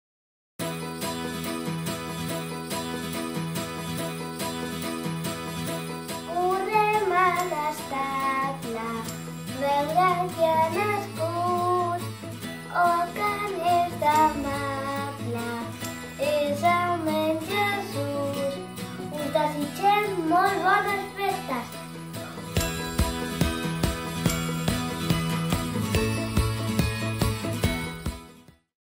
Promocional Vídeos de Nadal 2015 (Roger)
Vols felicitar aquestes festes de la forma més original? Grava't amb el mòbil o tablet (màxim 20" i en format hortizontal), envia'ns el resultat per Whasapp i l'emetrem per televisió. Optaràs a un magnífic premi consistent en un cap de setmana a l'Hotel La Mola. Ajuda'ns a felicitar les festes. Volem viure el aquestes festes ‪#‎apropteu‬